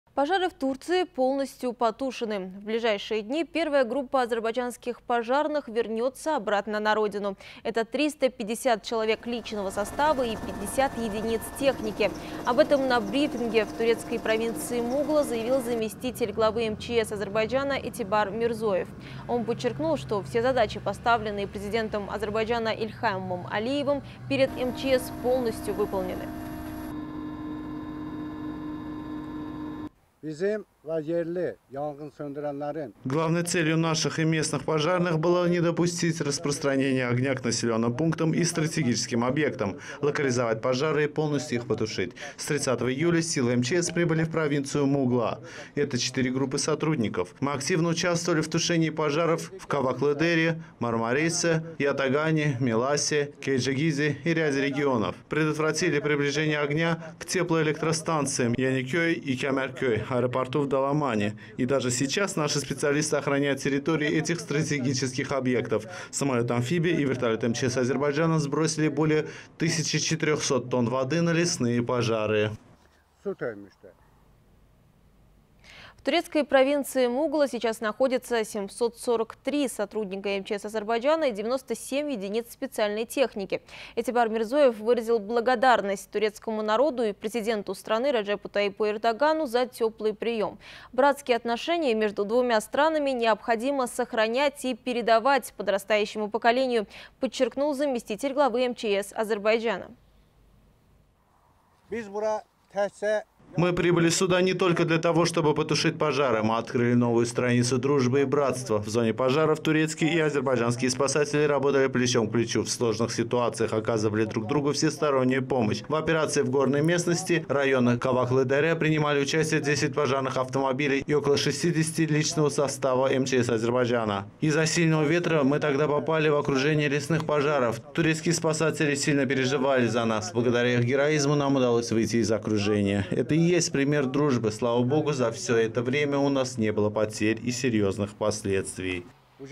Пожары в Турции полностью потушены!
В ближайшие дни первая группа азербайджанских пожарных вернется из Турции на Родину. Это 350 человек личного состава и 50 единиц техники. Об этом на брифинге в турецкой провинции Мугла заявил заместитель главы МЧС Азербайджана Этибар Мирзоев. Он подчеркнул, что все задачи, поставленные Президентом Азербайджана Ильхамом Алиевым перед МЧС, полностью выполнены.